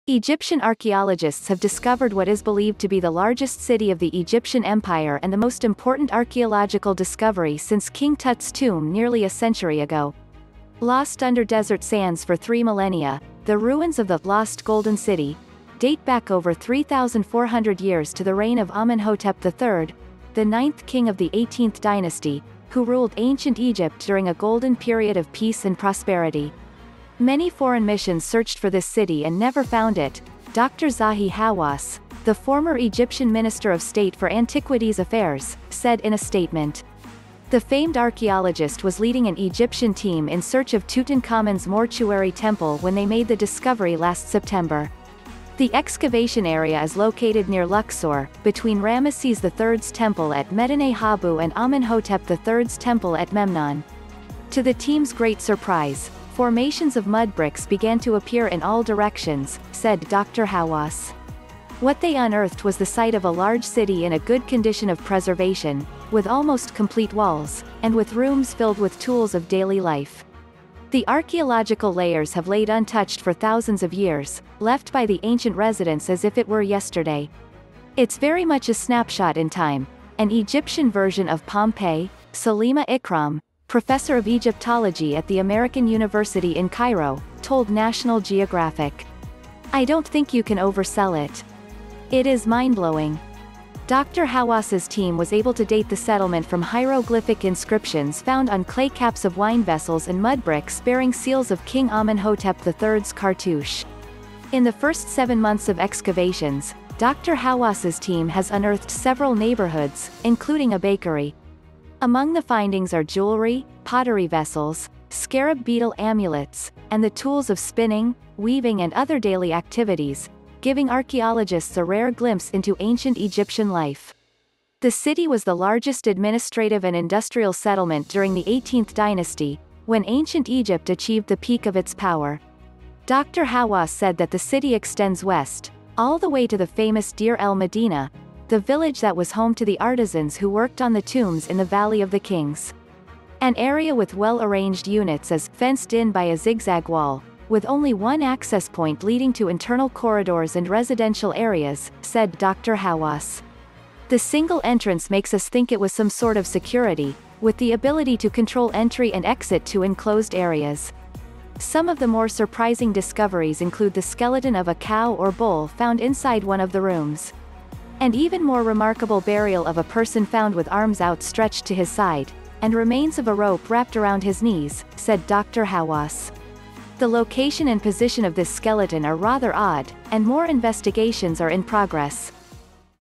Photos Egypt’s 3400 Year Old ‘Lost Golden City’ Is Unearthed From Desert
Egyptian archaeologists have discovered what is believed to be the largest city of the Egyptian empire and the most important archaeological discovery since King Tut’s tomb nearly a century ago. Lost under desert sands for three millennia, the ruins of the “Lost Golden City” date back over 3,400 years to the reign of Amenhotep III, the ninth king of the 18th dynasty, who ruled ancient Egypt during a golden period of peace and prosperity.“Many foreign missions searched for this city and never found it,” Dr. Zahi Hawass, the former Egyptian Minister of State for Antiquities Affairs, said in a statement. The famed archaeologist was leading an Egyptian team in search of Tutankhamun’s mortuary temple when they made the discovery last September. The excavation area is located near Luxor, between Rameses III’s temple at Medinet Habu and Amenhotep III’s temple at Memnon.“To the team's great surprise, formations of mud bricks began to appear in all directions,” said Dr. Hawass. “What they unearthed was the site of a large city in a good condition of preservation, with almost complete walls, and with rooms filled with tools of daily life. The archaeological layers have laid untouched for thousands of years, left by the ancient residents as if it were yesterday.”“It’s very much a snapshot in time—an Egyptian version of Pompeii,” Salima Ikram, professor of Egyptology at the American University in Cairo, told National Geographic. “I don’t think you can oversell it. It is mindblowing.”Dr. Hawass’s team was able to date the settlement from hieroglyphic inscriptions found on clay caps of wine vessels and mud bricks bearing seals of King Amenhotep III's cartouche. In the first seven months of excavations, Dr. Hawass’s team has unearthed several neighborhoods, including a bakery. Among the findings are jewelry, pottery vessels, scarab beetle amulets, and the tools of spinning, weaving and other daily activities, giving archaeologists a rare glimpse into ancient Egyptian life. The city was the largest administrative and industrial settlement during the 18th dynasty, when ancient Egypt achieved the peak of its power. Dr. Hawass said that the city extends west, “all the way to the famous Deir el-Medina,” the village that was home to the artisans who worked on the tombs in the Valley of the Kings. An area with well-arranged units is “fenced in by a zigzag wall, with only one access point leading to internal corridors and residential areas,” said Dr. Hawass. “The single entrance makes us think it was some sort of security, with the ability to control entry and exit to enclosed areas.”Some of the more surprising discoveries include the skeleton of a cow or bull found inside one of the rooms. “And even more remarkable burial of a person found with arms outstretched to his side, and remains of a rope wrapped around his knees,” said Dr. Hawass. “The location and position of this skeleton are rather odd, and more investigations are in progress.